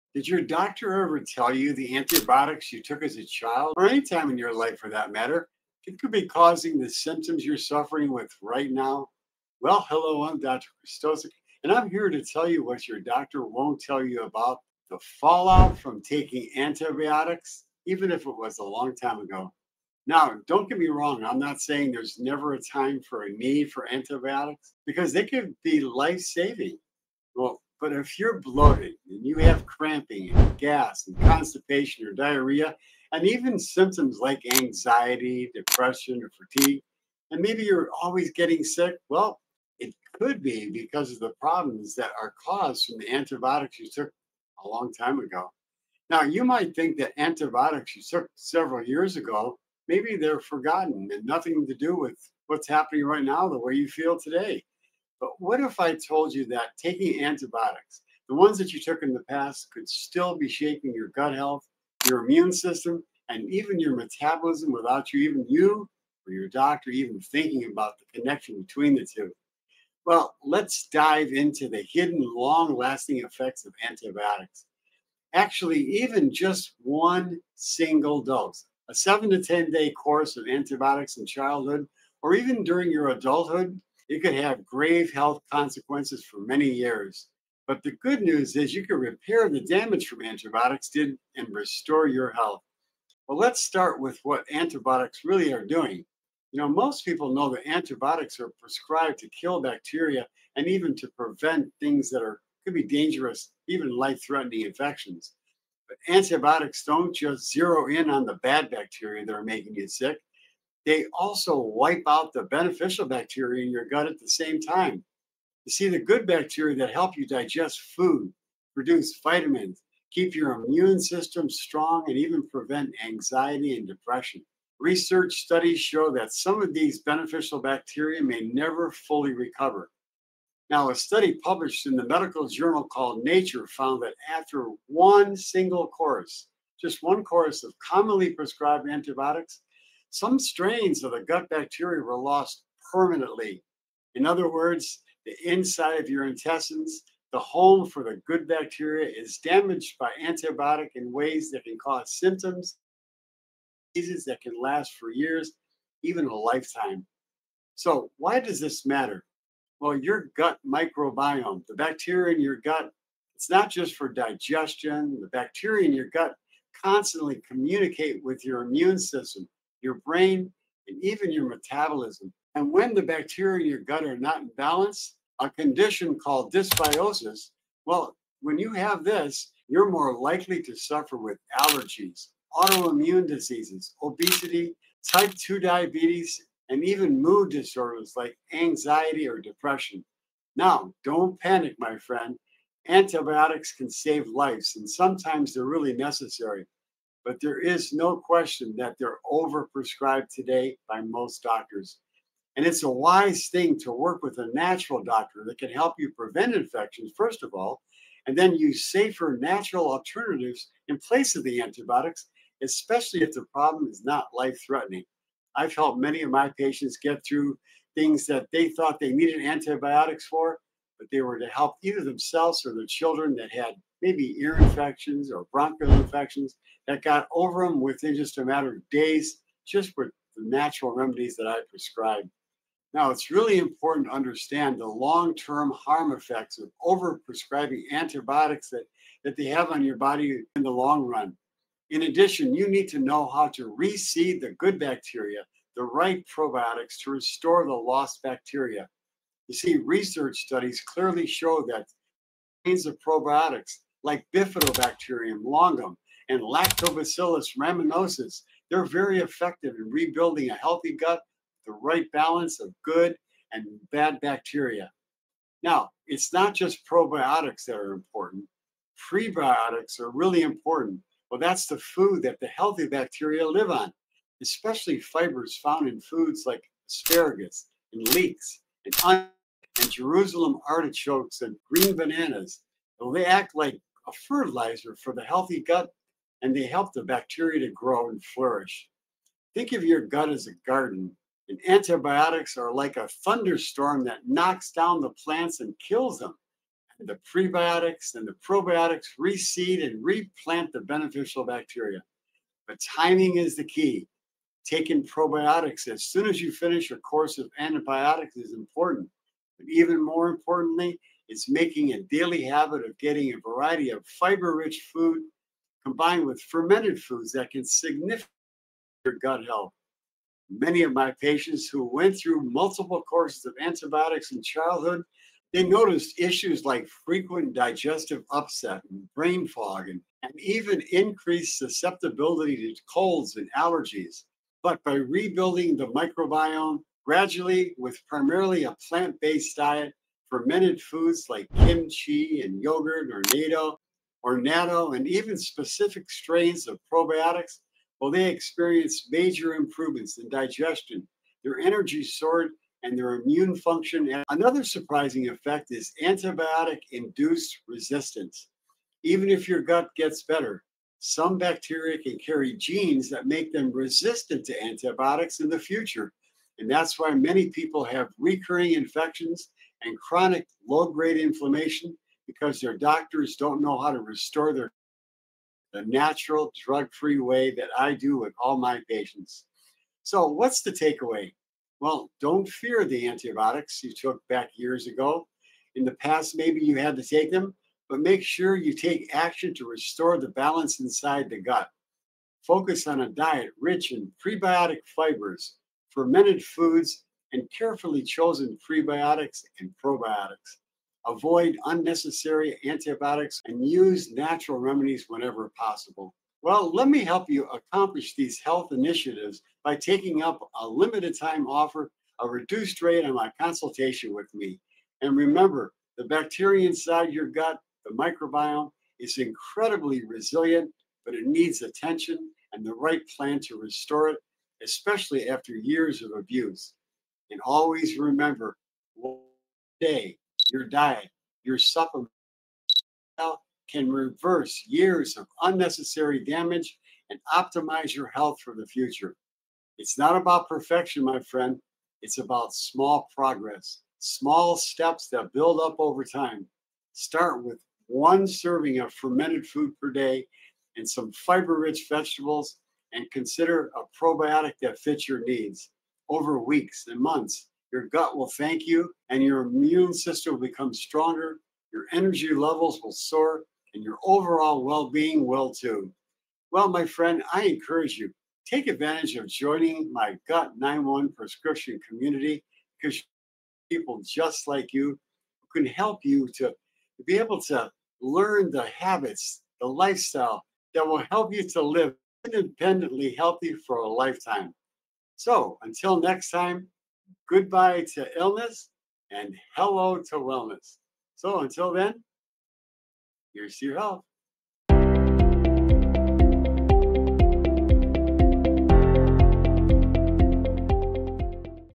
1 Simple Step to Reverse Antibiotics Damage and Restore Your Health
Did you know that antibiotics you took years ago could still be quietly affecting your gut, immune system, and metabolism? In this video, Dr. Krystosik reveals the hidden, long-term effects of antibiotics, explains how they disrupt your gut microbiome, and shares practical, evidence-based ways to restore gut health naturally.

You’ll discover:
How antibiotics damage beneficial gut bacteria permanently
The link between gut dysbiosis and chronic health issues like allergies, autoimmune disorders, and brain fog
The best probiotics and prebiotic foods to rebuild your microbiome
Tips for protecting your gut from future antibiotic exposure
How small daily habits can reverse years of unintended damage

Your gut health is the foundation of your overall wellbeing—don’t ignore the hidden consequences of past antibiotic use. Take action now to reclaim your health!